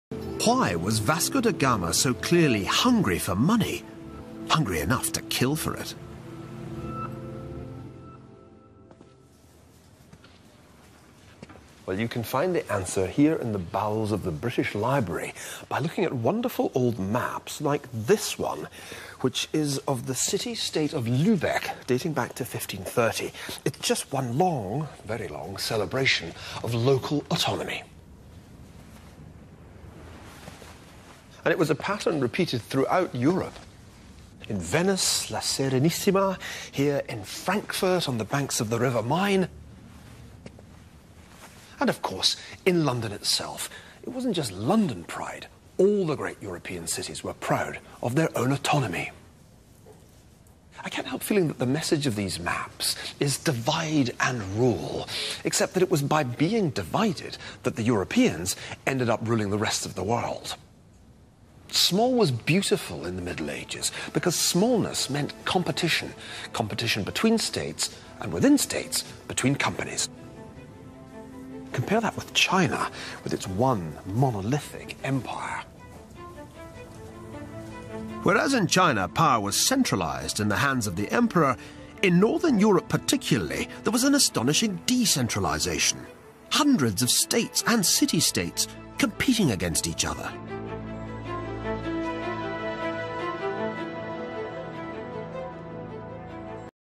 Our Diversity Is Our Strength (competition built the West)
How the diversity of Europe helped it dominate the globe.